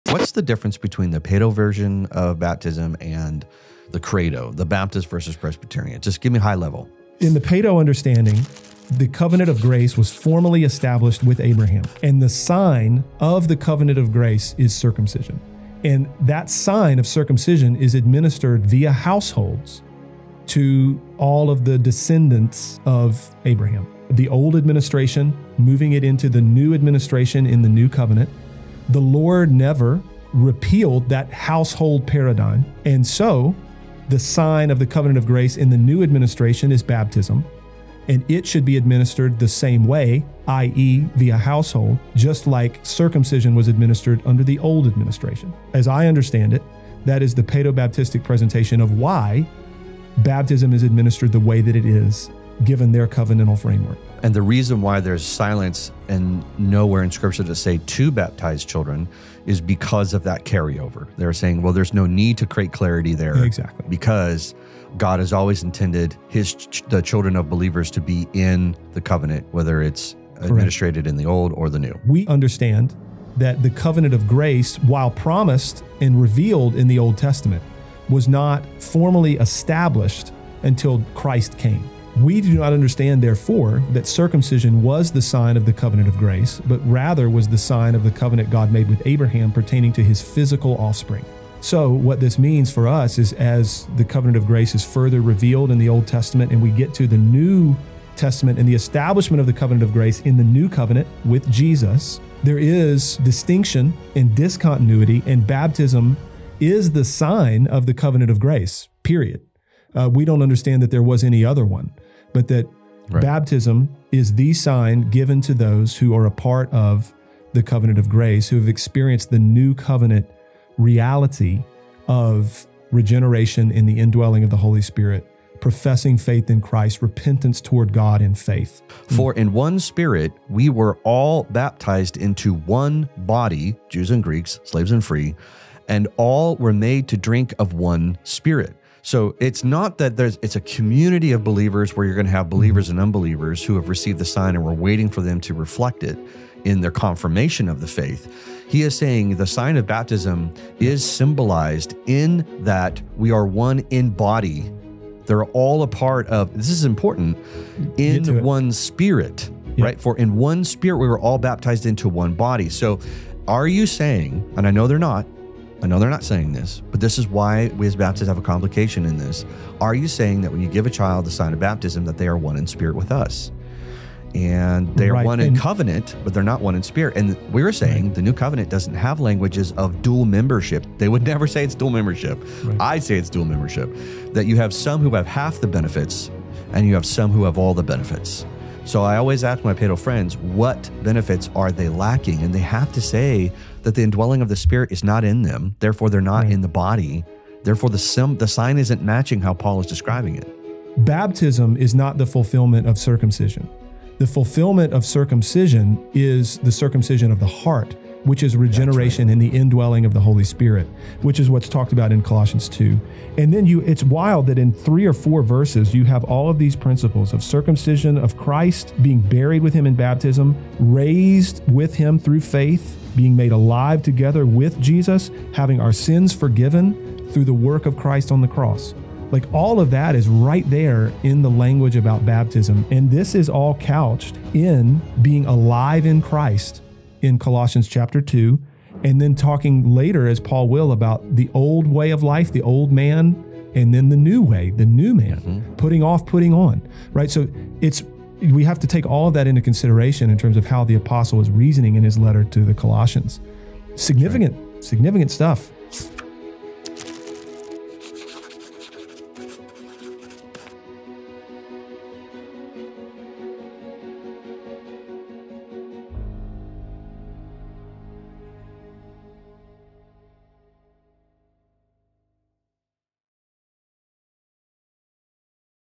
Why do Baptists and Presbyterians Differ on Baptism?
What is the difference between the Baptist and Presbyterian view on baptism? Jon and Justin lay the groundwork for the baptism debate in this short clip.